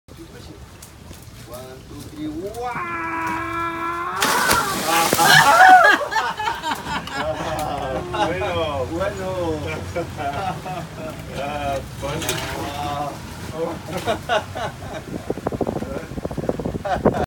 Costa Rica Crocodile
Crocodile jumps in water during a Costa Rica adventure.